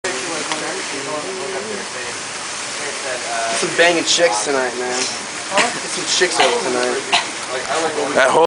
Robert stares
Robert thinks he is getting his picture taken...he just stares.  Just a starer.